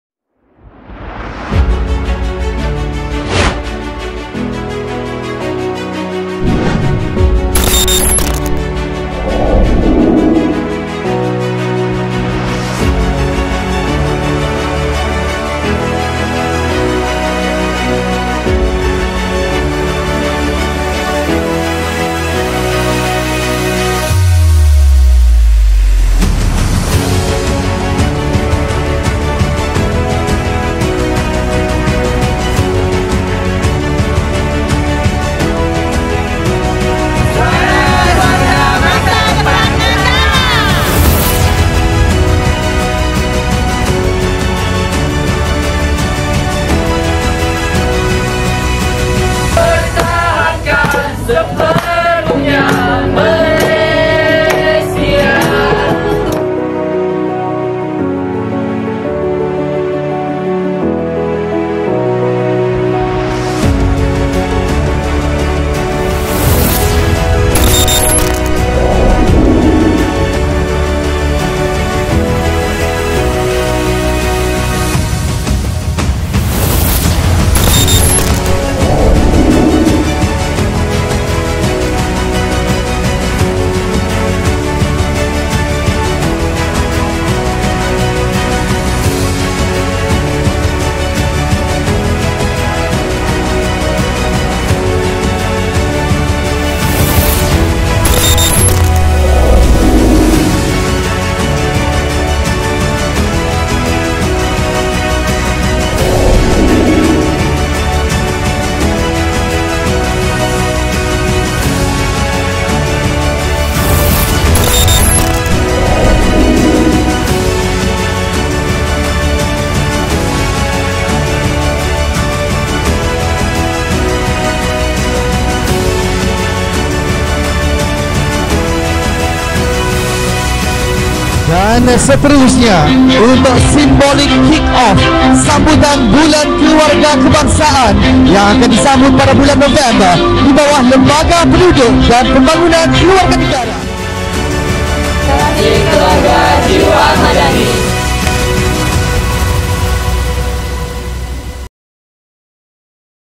KEMBARA KASIH KPWKM 2024
Sempena program Kembara KASIH KPWKM 2024, rakyat bumi kenyalang khususnya diluar bandar telah berpeluang menikmati perkhidmatan yang diberikan secara terus oleh Lembaga Penduduk dan Pembangunan Keluarga Negara (LPPKN) pada 14-21 September lalu.

Ketua Pengarah LPPKN YBrs. Abdul Shukur Abdullah berkata, platform kembara ini memberi pendedahan menyeluruh kepada pemimpin masyarakat serta rakyat khususnya di luar bandar mengenai perkhidmatan percuma disediakan LPPKN dan agensi lain di bawah KPWKM.

Kembara sejauh 1,456km ini, telah merentasi 8 daerah bermula di Kuching dan berakhir di Lawas. Semoga usaha ini memberi manfaat buat warga Sarawak.